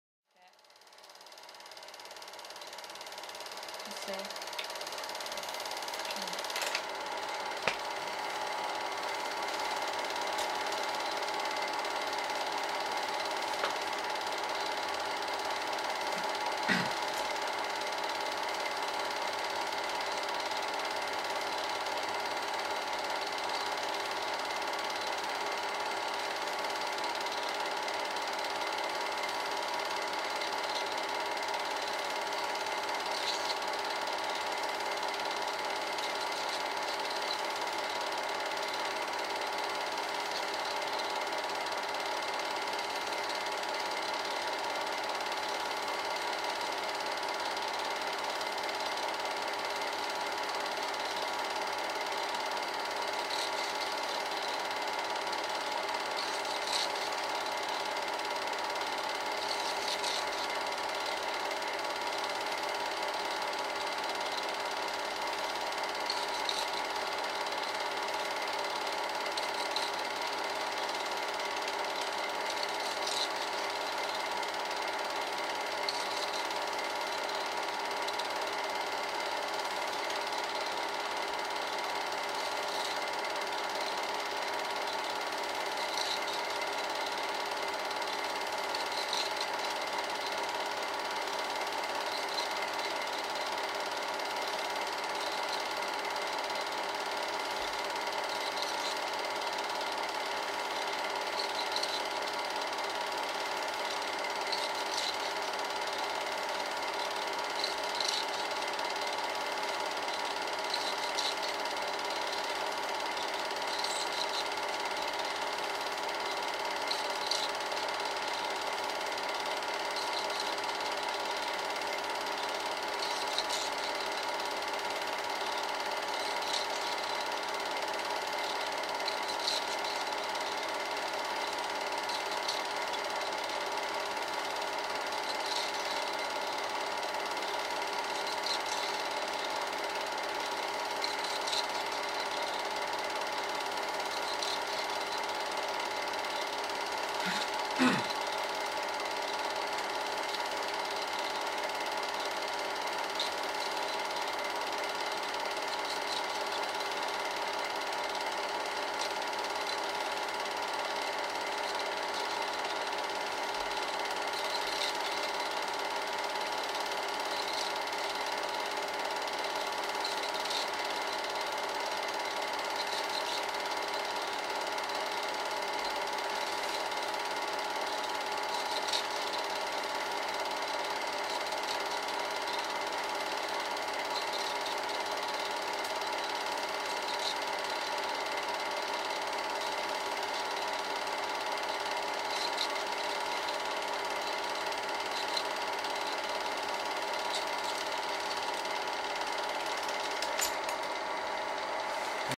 8mm
Short film shot on Super 8 film. (Self-processed)
By Jessica Melling
BA Fine Art, Kingston University.